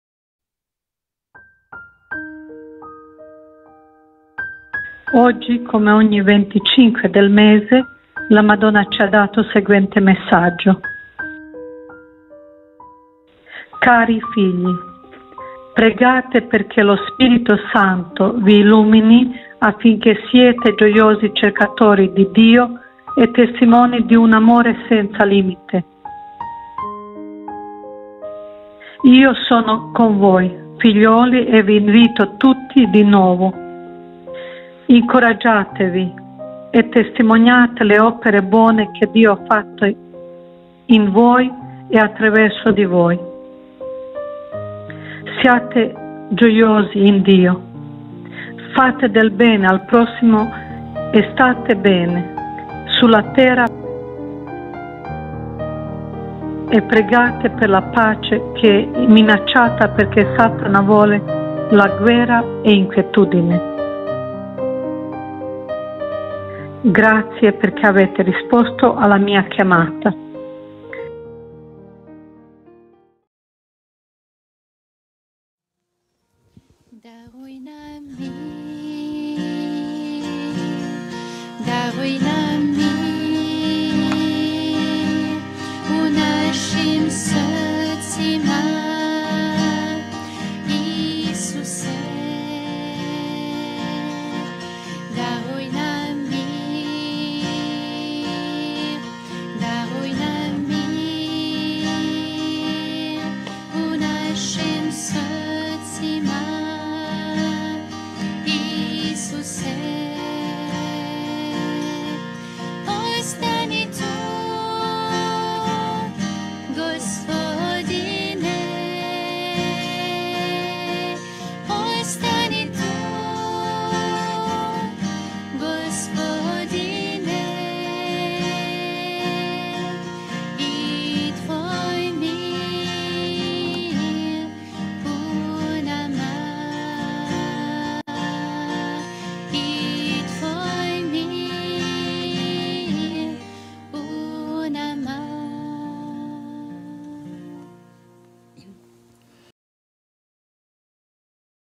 A PAZ ESTÁ AMEAÇADA - Medjugorje Mensagem
▶ Aparição de Nossa Senhora Rainha da Paz em Medjugorje para a vidente Marija Pavlovic
▶Mensagem de Nossa Senhora em Medjugorje
▶Mensagem para a humanidade
▶Mensagem de hoje